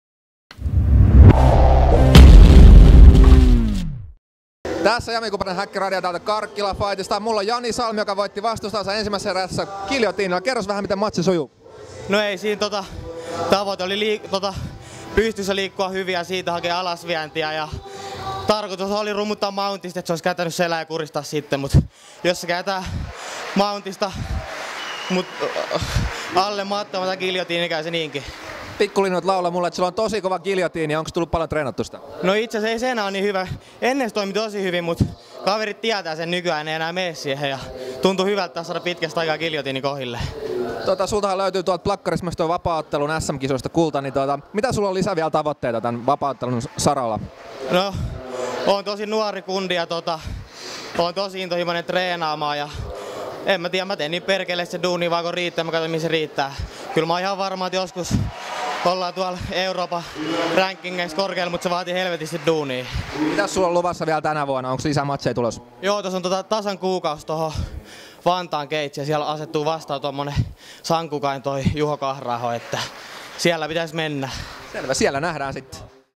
Karkkila Fight Night - Jani Salmi
B73kg
Jani Salmi ( 1-2-0 ) GB Gym / Team MMA Finland vs Kalle Arvelin ( 0-4-0 ) Shooto Lahti
- Salmi, giljotiinikuristus, erä 1 1:17